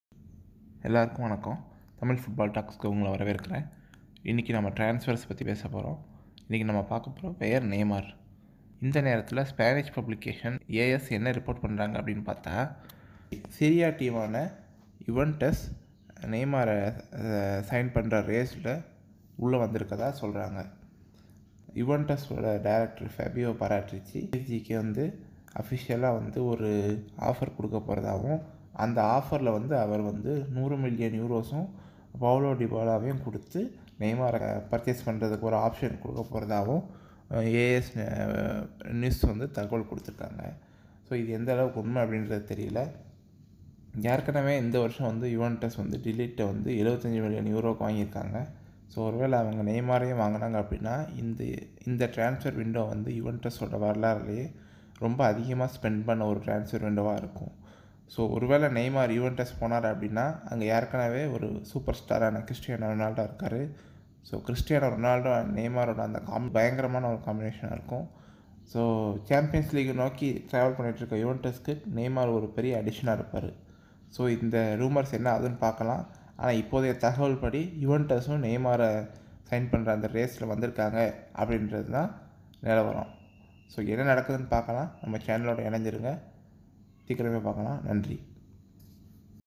Neymar Latest Transfer News:Neymar Going To Juventus?(Tamil)
Neymar Latest Transfer News:Juventus Are In The Race To SIgn Neymar According To AS.

நெய்மர் சமீபத்திய  செய்திகள்: ஜுவென்ட்ஸ் செல்கிறார் நெய்மார் ?